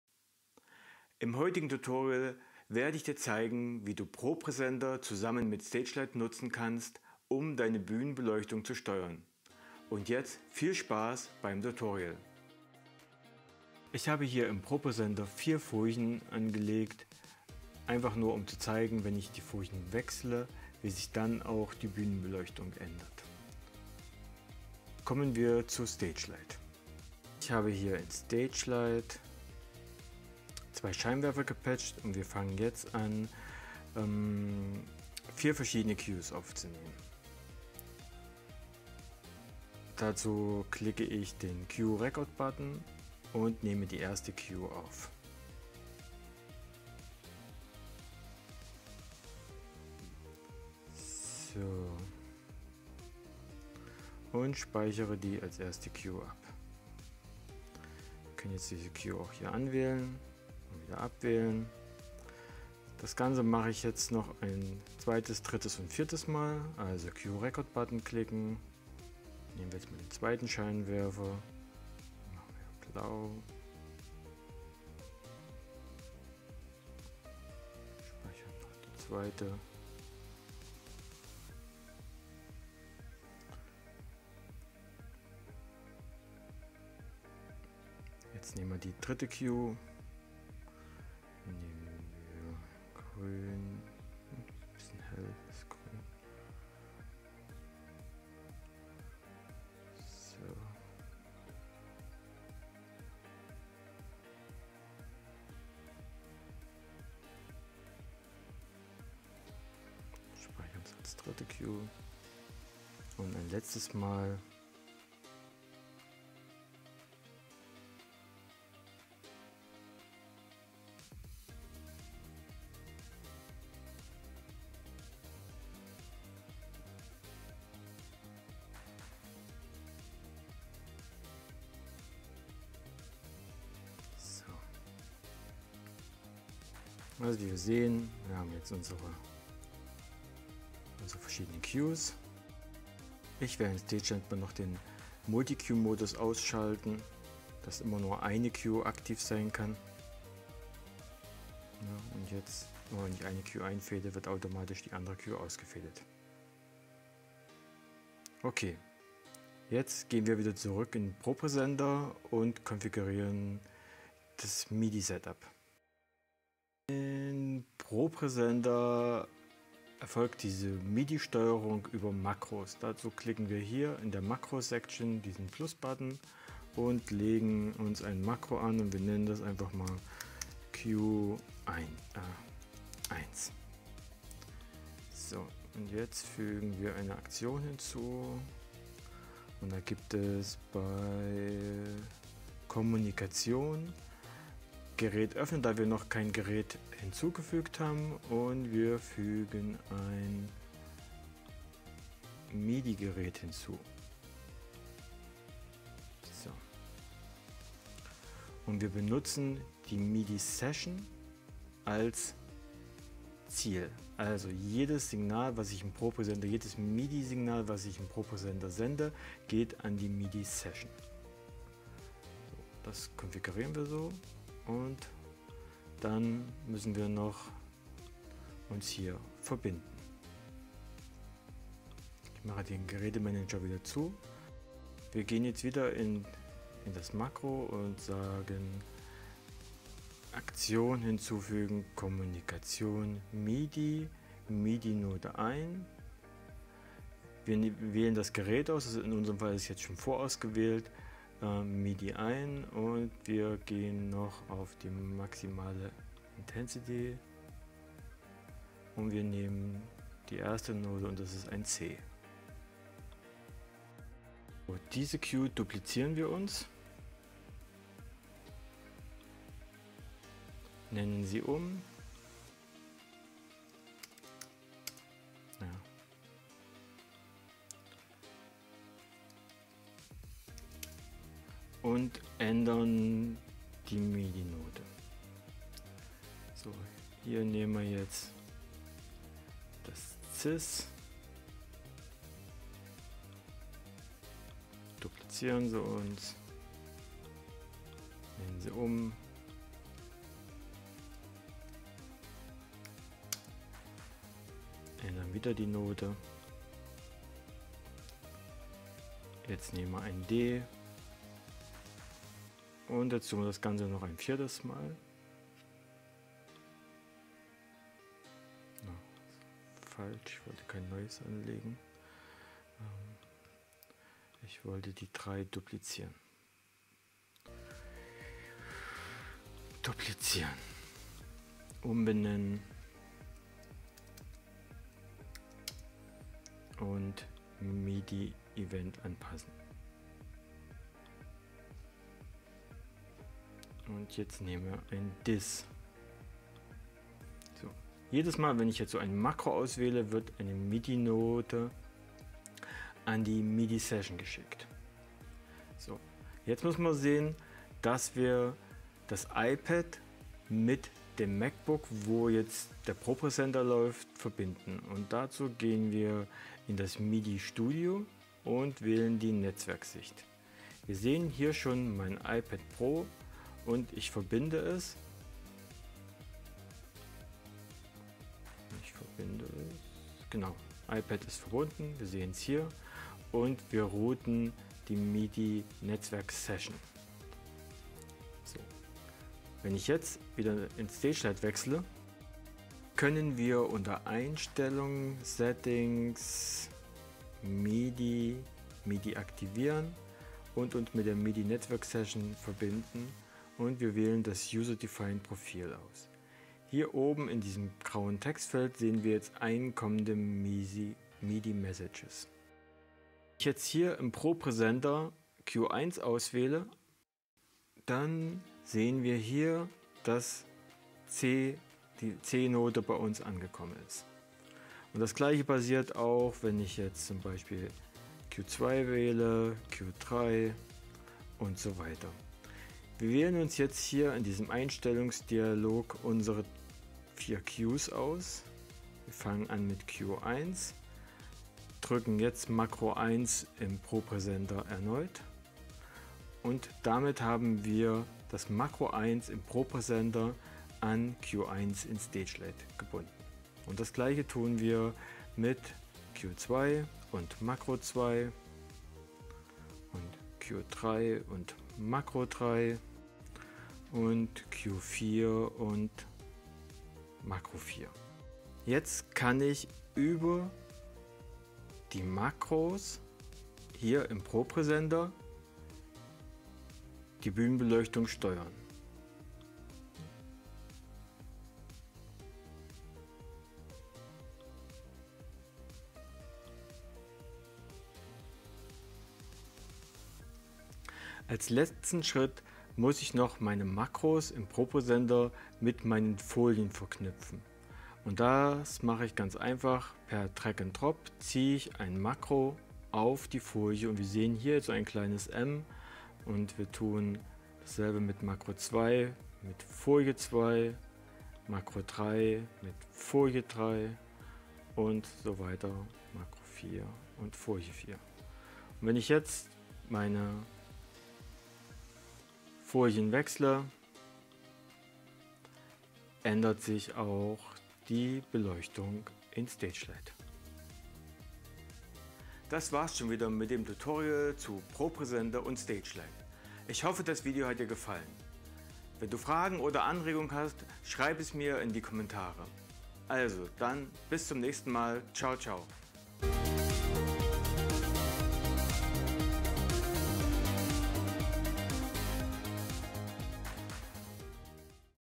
ProPresenter & StageLight (Deutsch, English subtitles)
In this tutorial, I want to show how to use the ProPresenter together with StageLight to control your lights in sync with the presentation or show.

StageLight - and the stage lighting becomes smart

StageLight is a professional lighting control app for the iPad.The software combines the classic hardware lighting console feeling with a modern and intuitive design. With StageLight have lighting designers and operators, musicians and artists, film and video makers an advanced control of lightings based on Art-Net (DMX over IP), and on CRMX (Wireless DMX).